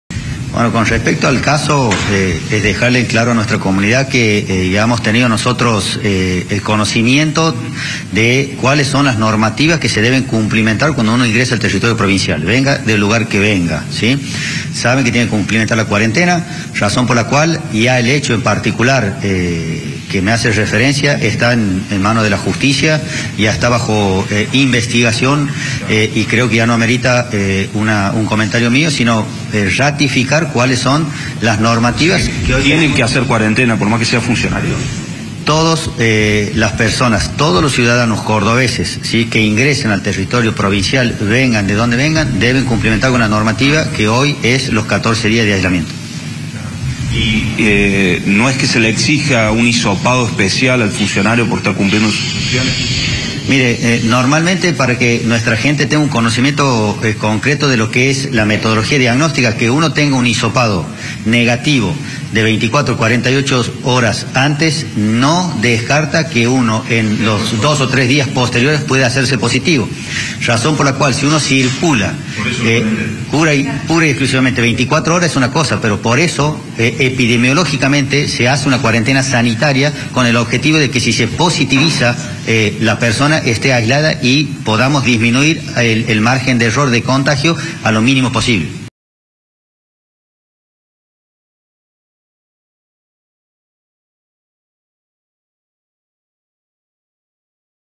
CORONAVIRUS | "TODOS DEBEN HACER CUARENTENA" - Diego Cardozo.
Martín Gill dio positivo y podría ser imputado. El funcionario nacional debería haber cumplimentado la cuarentena al ingresar a la provincia. Habla el ministro de Salud de Córdoba, Diego Cardozo.